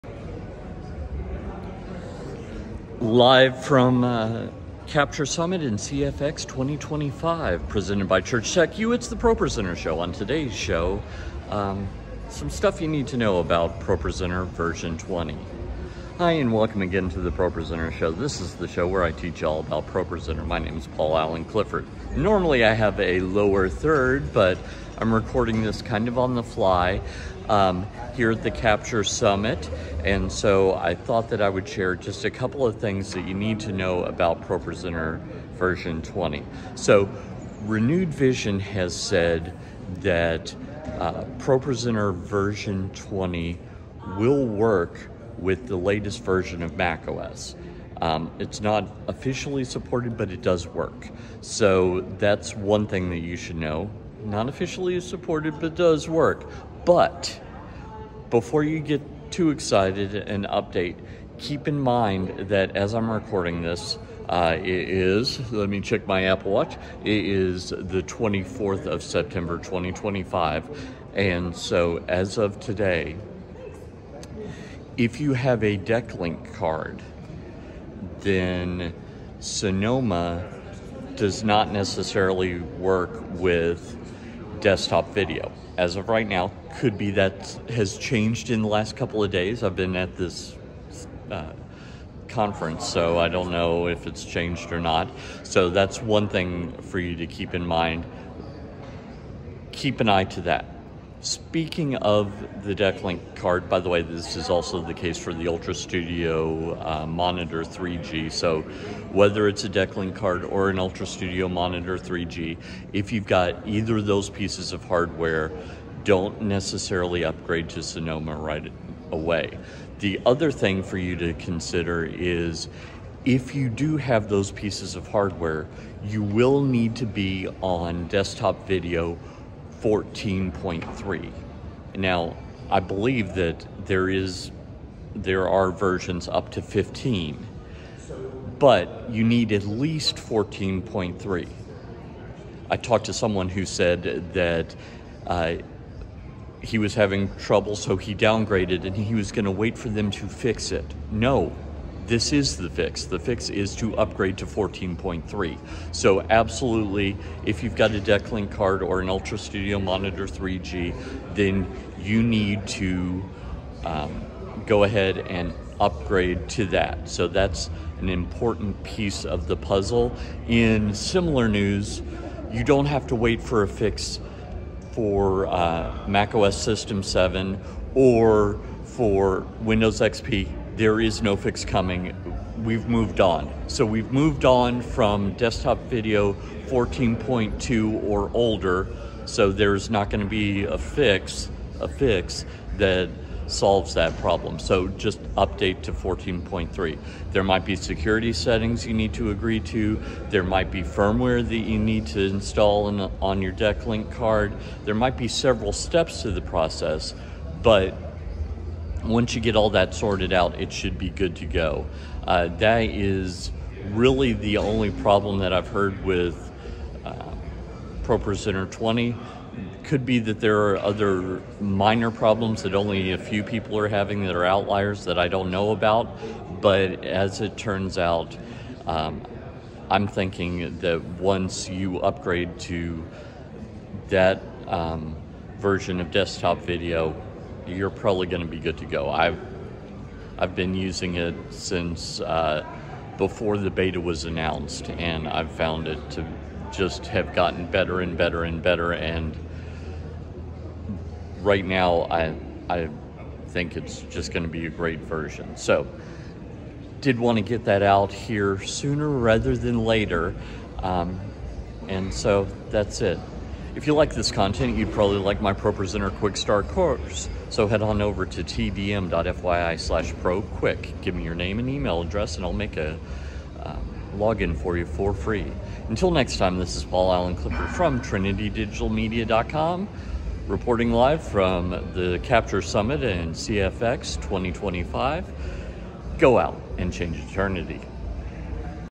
What you need to know about upgrading to ProPresenter v20
Bullet points: MacOS Tahoe should be fine BUT other hardware and software might have issues, especially desktop video from BlackMagic Design, if you have a decklink card or
Ultrastudio monitor 3g.
Also, 14.3 of BlackMagic’s Desktop Video software (or newer) is required;
I'M USING DESKTOP VIDEO 14.5 WITH MACOS SEQUOIA 15.6.1.**

THIS IS NO UPDATE COMING TO ALLOW AN OLDER VERSION OF DESKTOP VIDEO THAN 14.3.